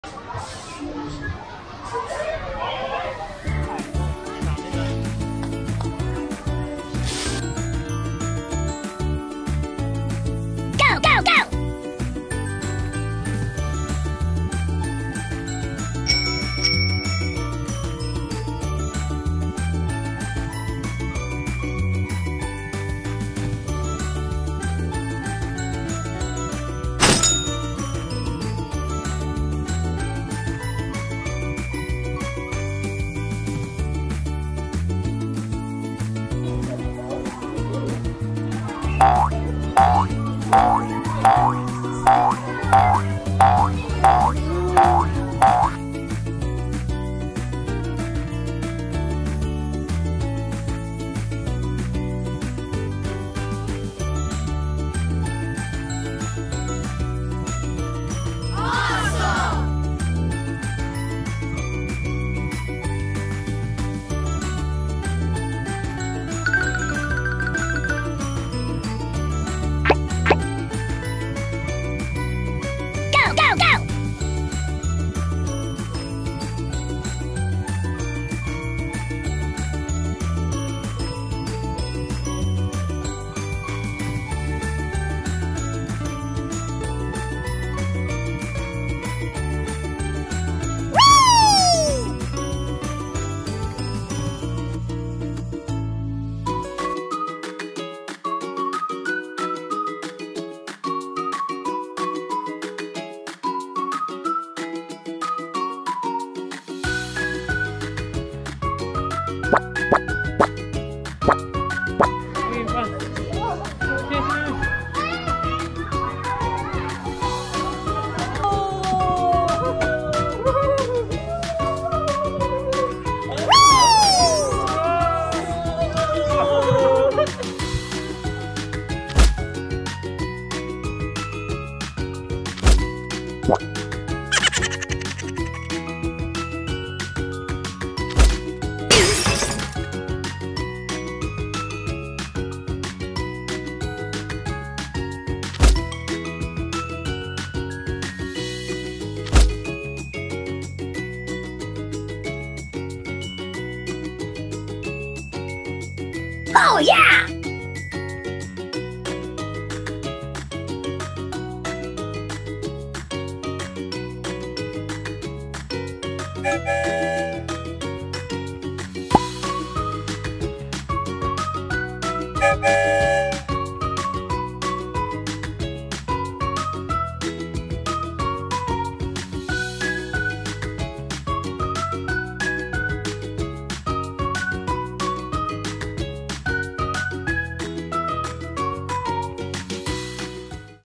Kiztopia Marina Square | OMG It's so CROWDED here | Singapore Indoor Playground
It's playdate time.. Jonas goes to Kiztopia at Marina Square Singapore with Kaley but OMG it is so crowded there.